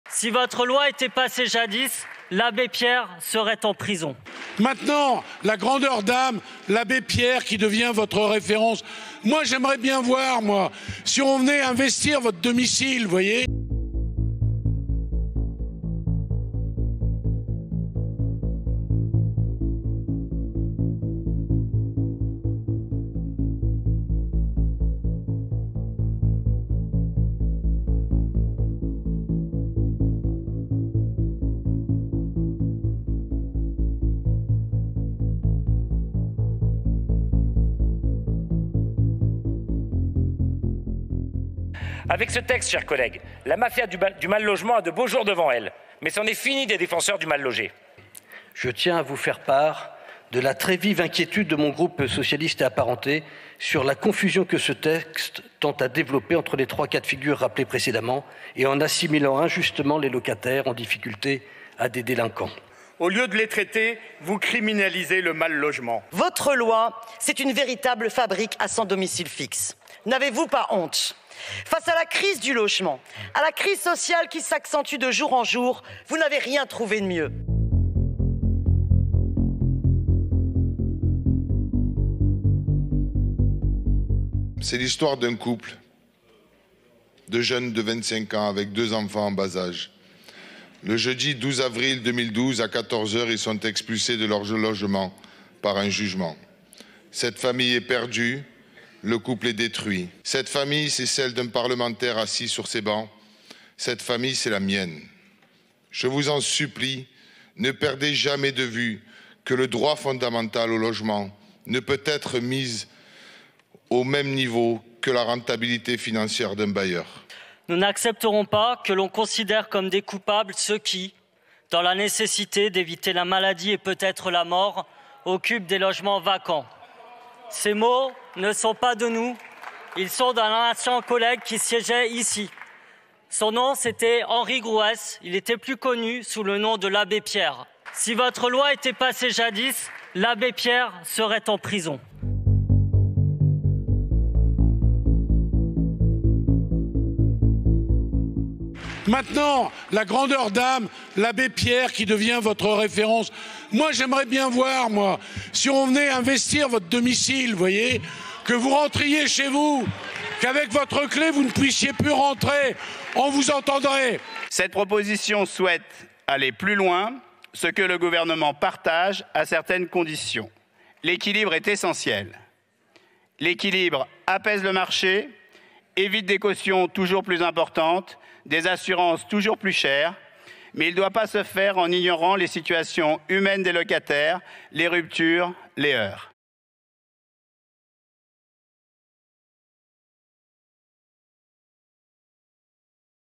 La proposition de loi anti-squats, soutenue par le gouvernement, indigne la gauche
POLITIQUE - « Si votre loi était passée jadis, l’abbé Pierre serait en prison ». Dans l’hémicycle, les députés de gauche se sont insurgés ce lundi 28 novembre. En cause, l’examen d’une proposition de loi anti-squats.

L’initiateur du texte, le macroniste Guillaume Kasbarian, a insisté à l’ouverture des débats sur les « petits propriétaires qui ne roulent pas sur l’or » et se retrouvent « victimes de squats ». Porté par les groupes Renaissance et Horizons, le texte propose de tripler les sanctions encourues par les squatteurs jusqu’à 3 ans de prison et 45 000 euros d’amende, et d’accélérer les procédures d’expulsion.

Dans une ambiance électrique, comme vous pouvez le voir dans notre vidéo en tête d’article, l’insoumis François Piquemal a défendu en vain une motion de rejet en s’indignant d’une « attaque sans précédent à l’encontre des droits et de la protection des locataires », par la « criminalisation de tous les mal-logés ».

Le député LFI Sébastien Delogu a raconté sa propre histoire « d’expulsion » en 2012, son « couple détruit » et son « parcours du combattant ».

Député d’Eure-et-Loir, Guillaume Kasbarian avait déjà fait voter en 2020 des dispositions contre les squats via un projet de loi, en partie censurées par le Conseil constitutionnel. Sa proposition prévoit aussi d’alourdir les sanctions pour ceux qui se font passer pour un propriétaire dans le but de louer un bien.

Le texte réduit la durée de la procédure judiciaire, et prévoit que certains délais pouvant être accordés par un juge ne soient désormais envisageables que si le locataire en fait lui-même la demande. « Une atteinte manifeste à l’office du juge de l’expulsion » pour la gauche, qui craint un manque de recours des locataires.

En l’absence de majorité absolue, le camp présidentiel devrait se tourner vers sa droite. « Ça fait bien longtemps qu’on fait des propositions allant dans ce sens », souligne la députée Annie Genevard (LR).

Sous les protestations, le RN Jean-Philippe Tanguy a lancé que le texte de Guillaume Kasbarian « s’inspirait largement » d’une proposition de Marine Le Pen.

Quant au gouvernement, s’il a apporté son soutien, il a prudemment demandé un « équilibre » et pourrait peser en séance. « L’assimilation entre un squatteur et un locataire mauvais payeur n’a pas lieu d’être », a ainsi insisté le ministre du Logement Olivier Klein. Son collègue Garde des Sceaux Éric Dupond-Moretti a largement défendu le texte, tout en étant « un peu plus réservé » quant aux « difficultés constitutionnelles » de l’article 2 qui étend la notion de domiciles à ceux vides de meubles.

Les débats devraient durer jusqu’en milieu de semaine.